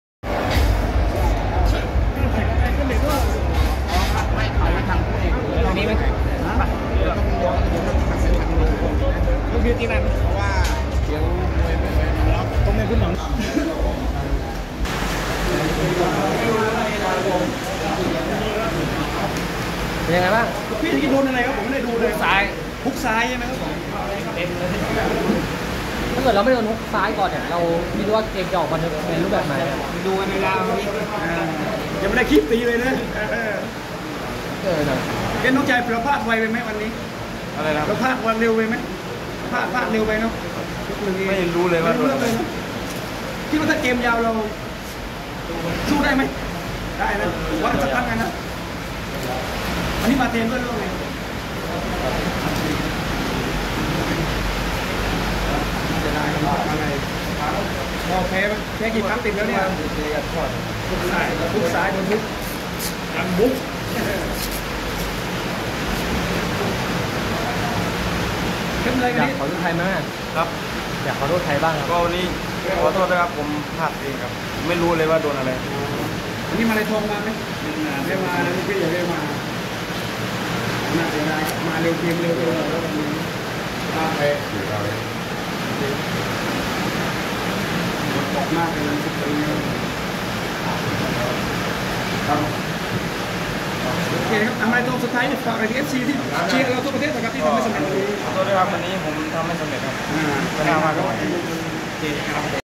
เกิดอะไรขึ้น!! TKO ยกแรกเลยหรอ!!? สัมภาษณ์หลังเวที เพชรมาลัย เพชรเจริญวิทย์
TKO ยกแรกเลยหรอ!!? สัมภาษณ์หลังเวที เพชรมาลัย เพชรเจริญวิทย์